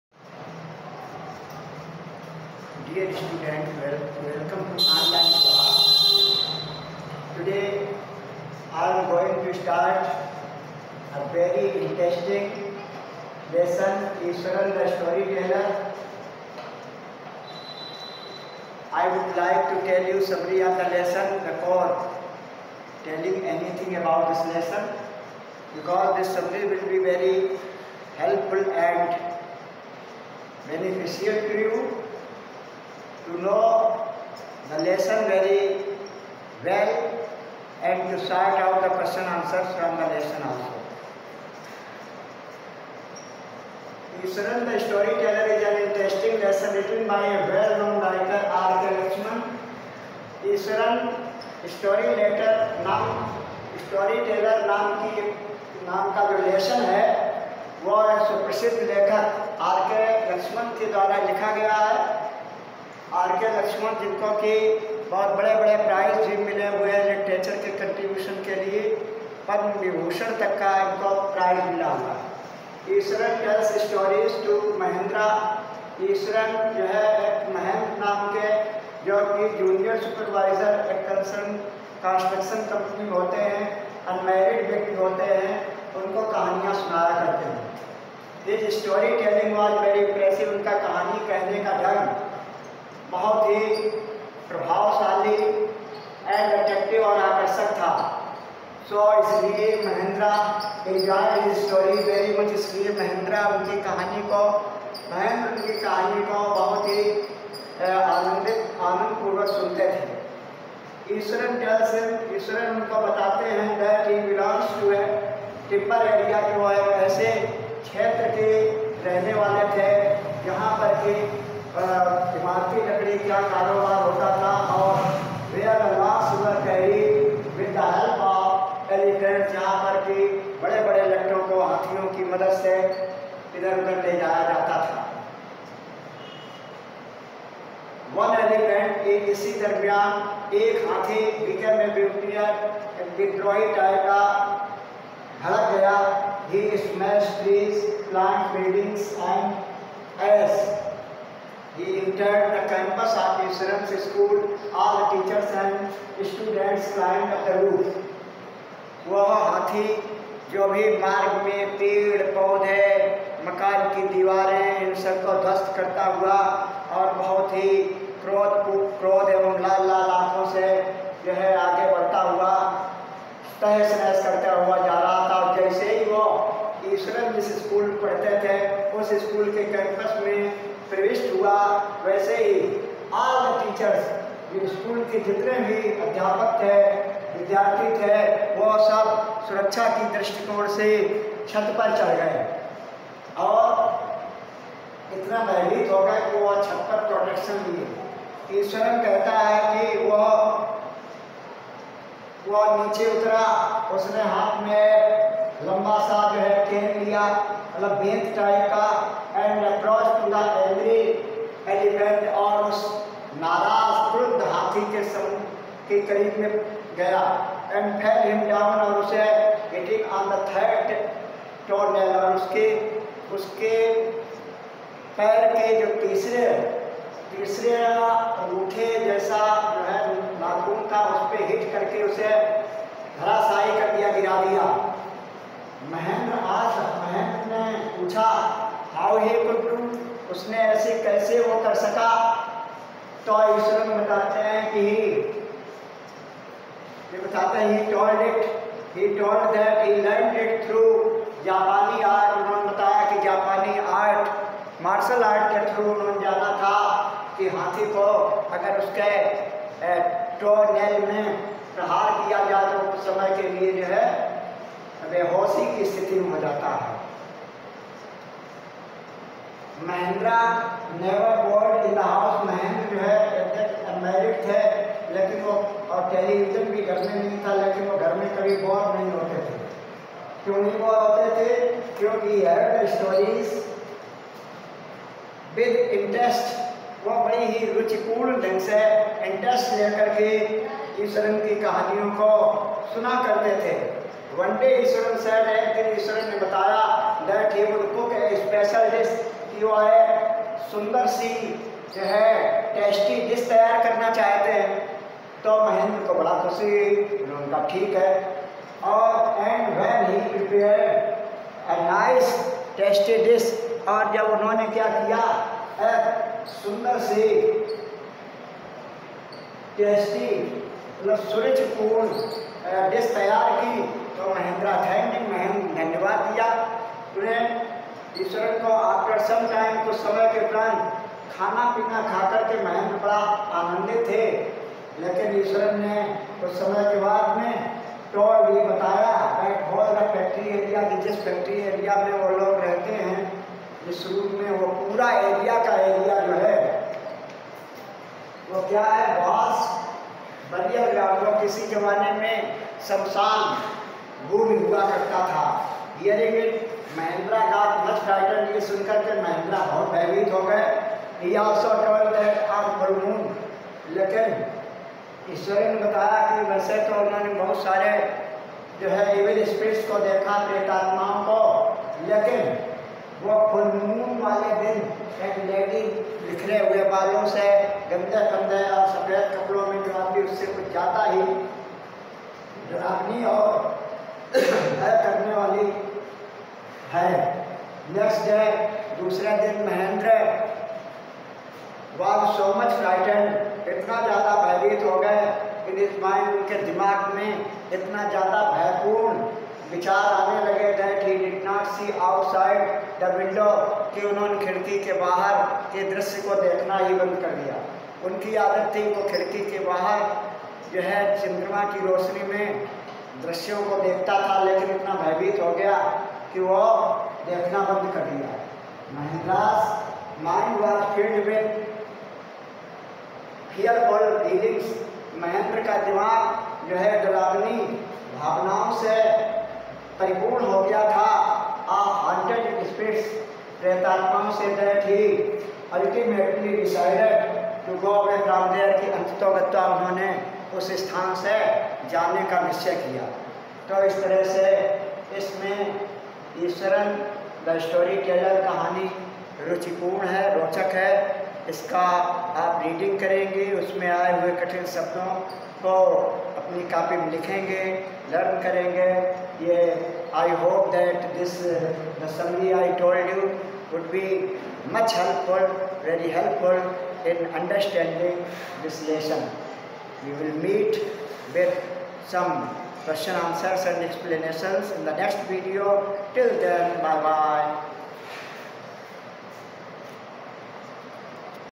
Class: 9 Sub:Eng.Lang.Topic:Iswaran, the Story teller
taught by Subhash Sir 27Aug.2020